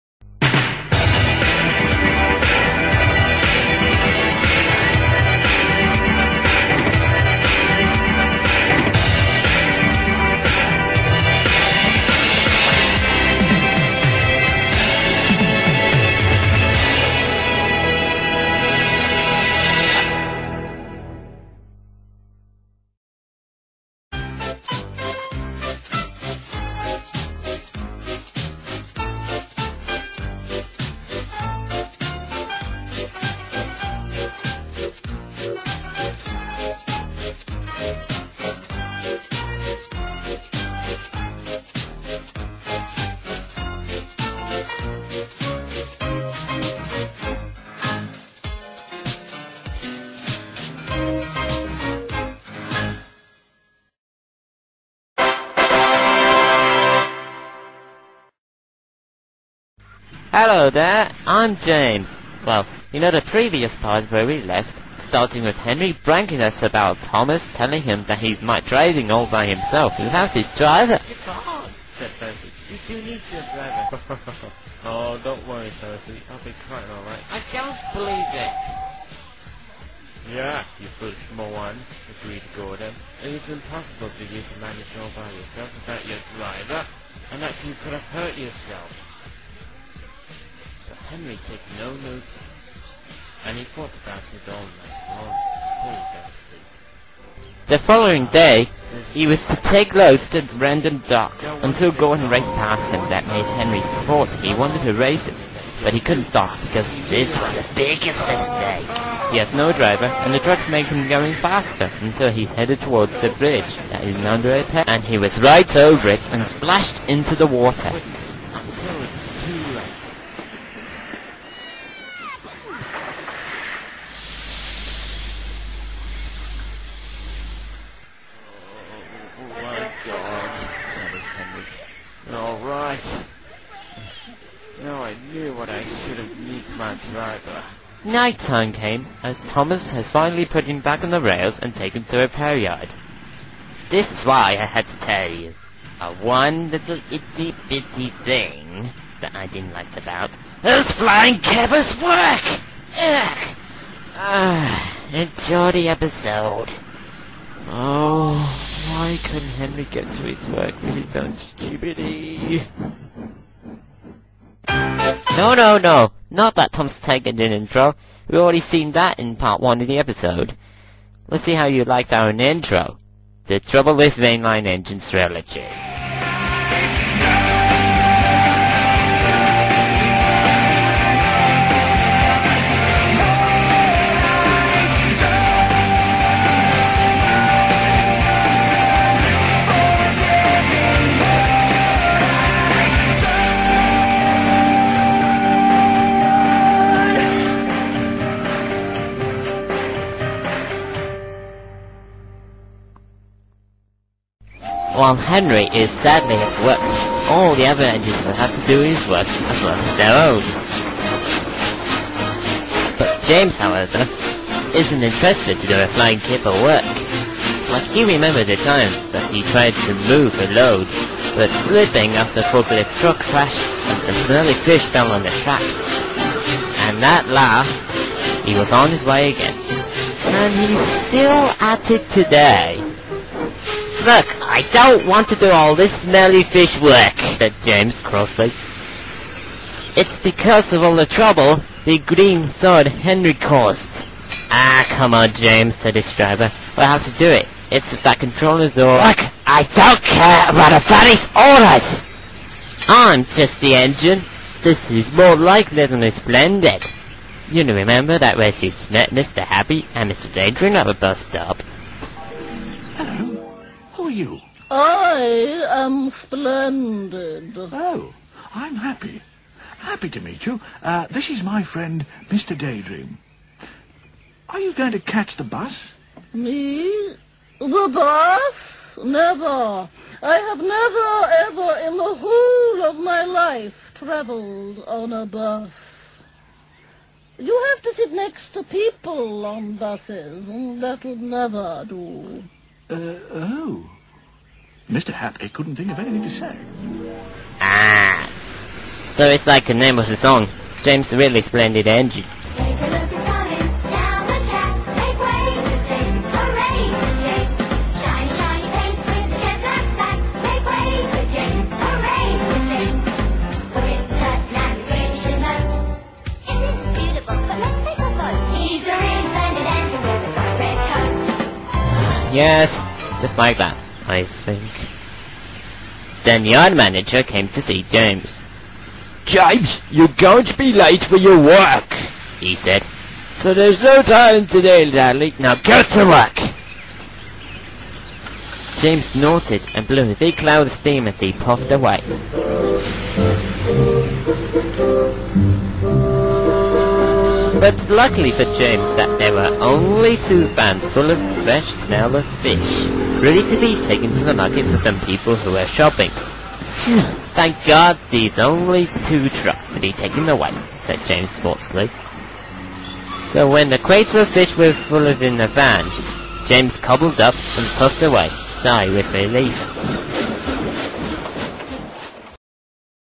The Trouble of Main Line Engines (Part 2) AKA James the Clown Engine (1)
Part two of Episode 5 of EAOTTTE! (Part 1)

After Henry was going to be repaired soon, James was angry about Henry's Work and wished to finsh the job.

Dan5589

NOTE: All Things were Copyrighted to their Respected Owners